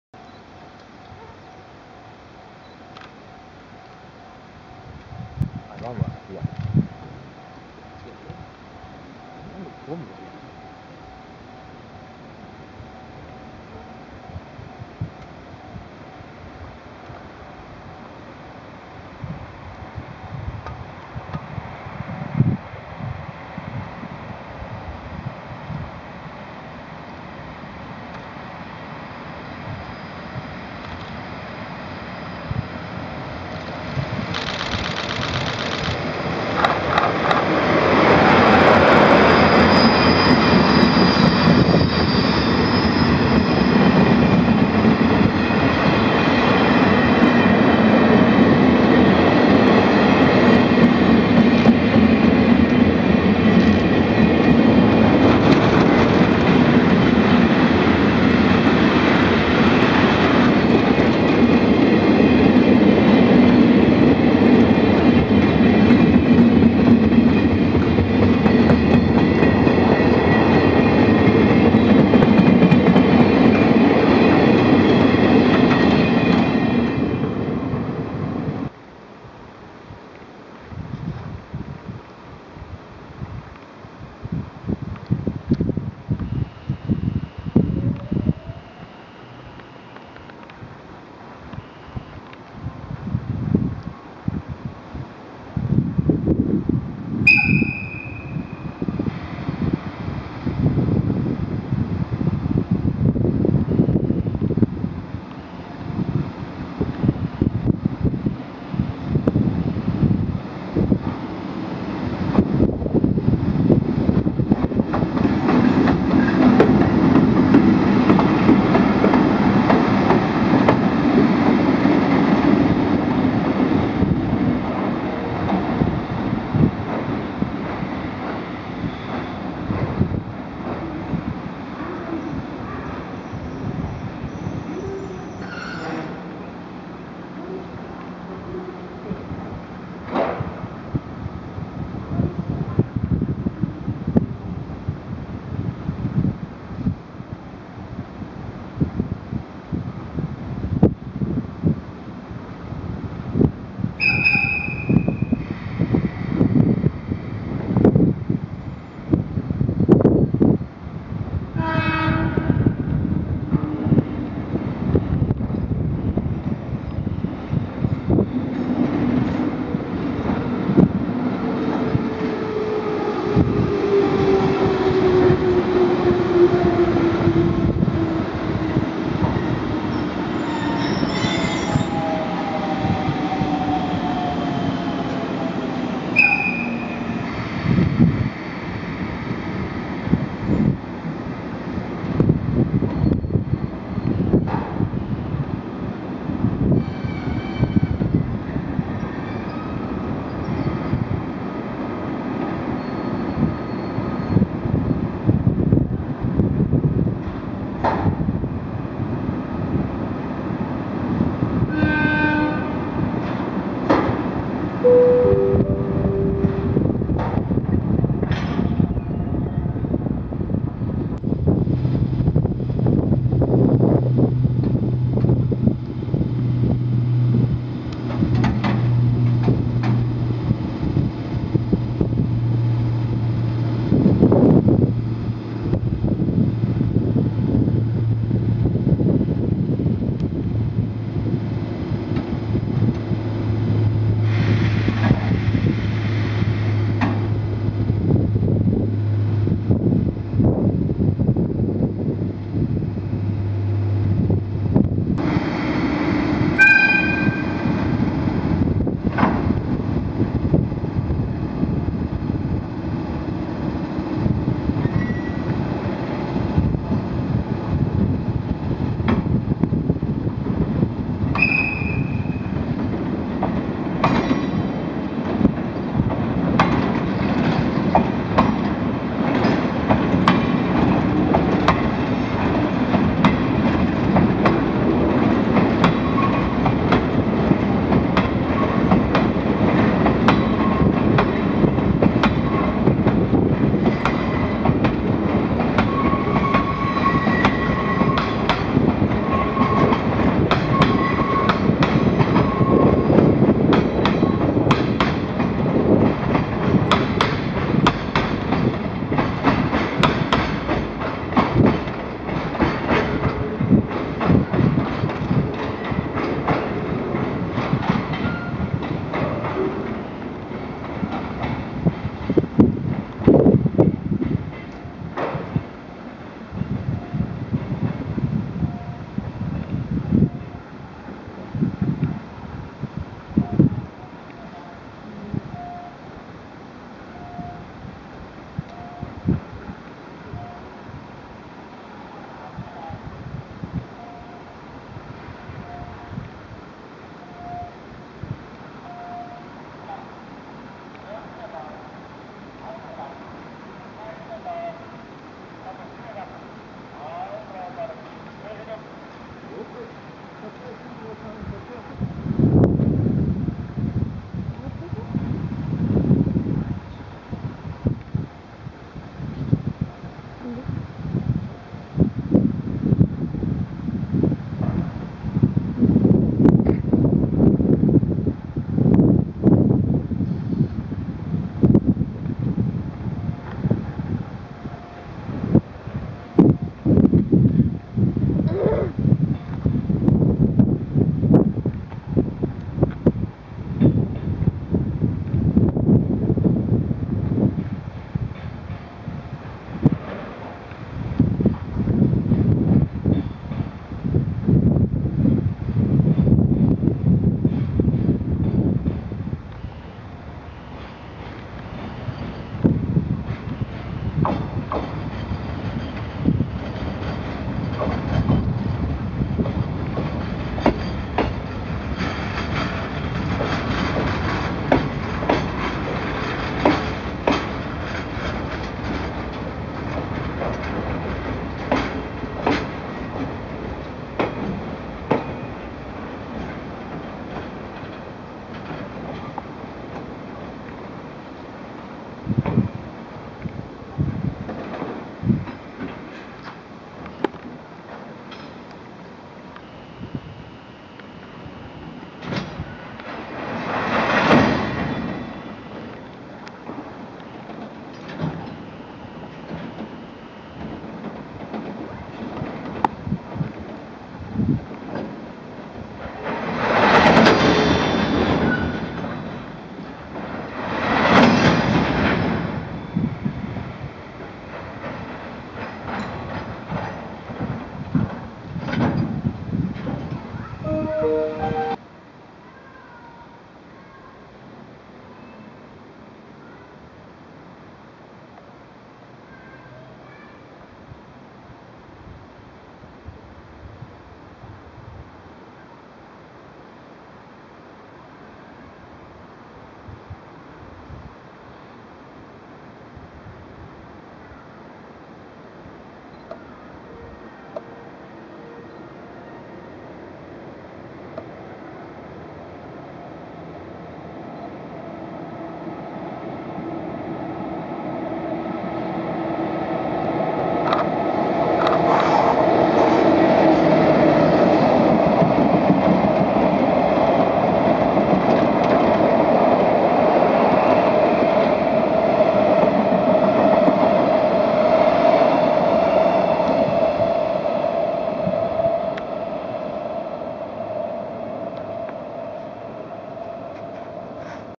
ワム80000貨物・東海道本線6170レ中央西線670レ
名古屋地区のワム80000輸送シーンです。
掲載内容は、
6170レ東海道本線・岐阜～木曽川　ワム37両！！
春日井駅出発準備
670レ中央西線・春日井～勝川